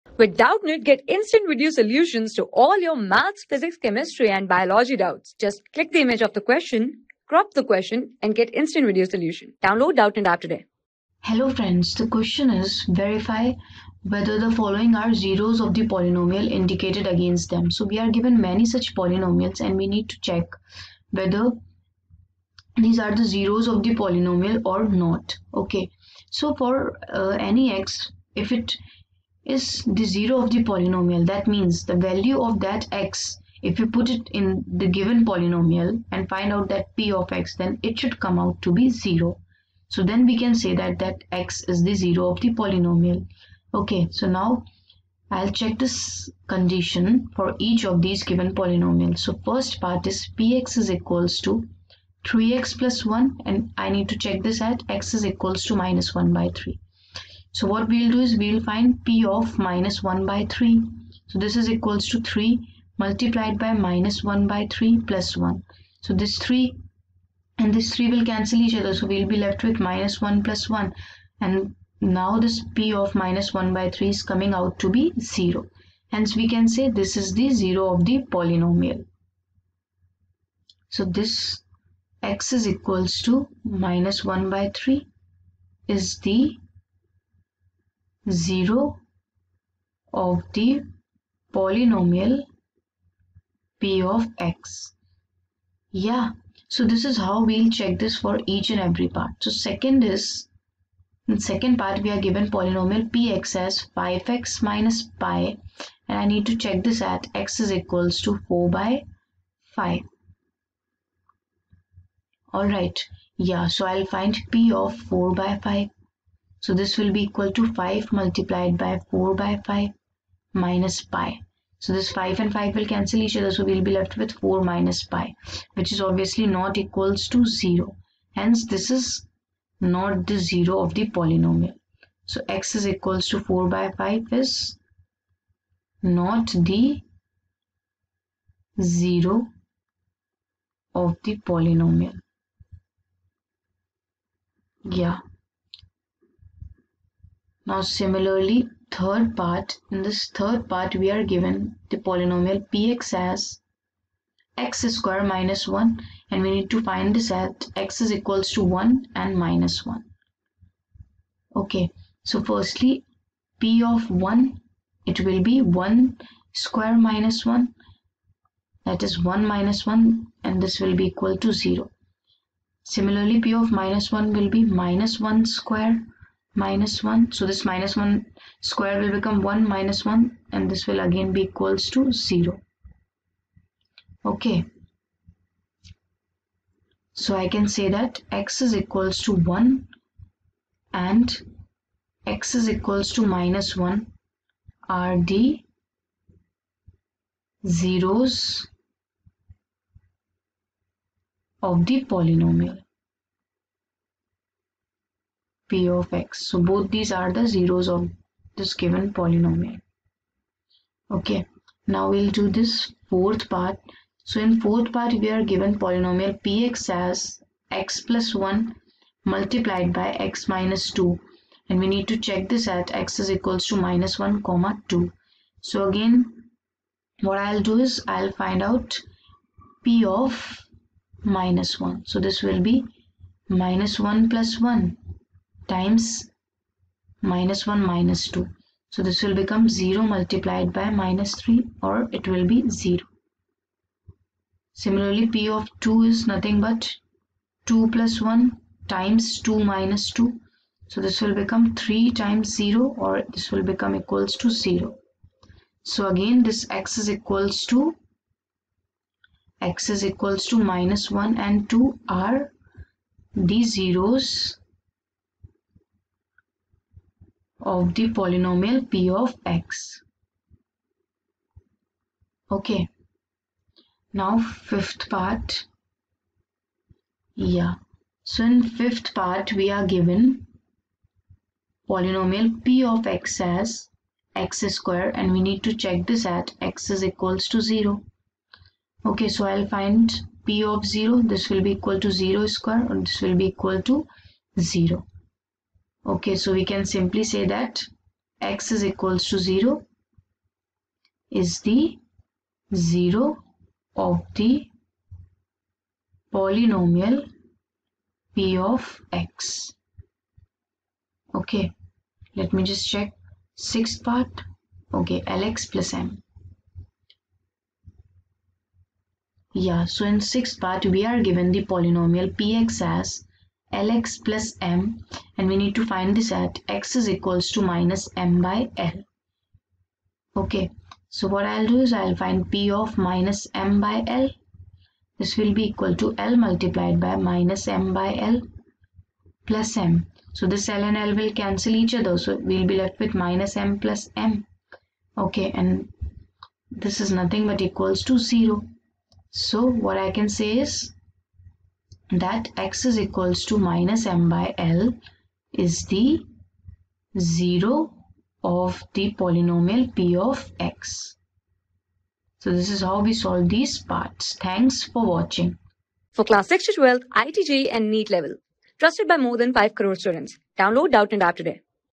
Verify whether the following are zeroes ofthe polynomial, indicated against them.(i) p(x)=3x+1, ...
Verify whether the following are zeroes ofthe polynomial, indicated against them.(i) p(x)=3x+1, x=-1/3 (ii) p(x)=5x-pi, x=4/5 (iii) p(x)=x^2-1, x=1,-1(iv) p(x)=(x+1)(x-2), x=-1,2 (v) p(x)=x^2, x=0 (vi) p(x)=l x+m, x=-m/l
Class: 9
Subject: MATHS
Chapter: POLYNOMIALS
Board:CBSE

👉 Have Any Query? Ask Us.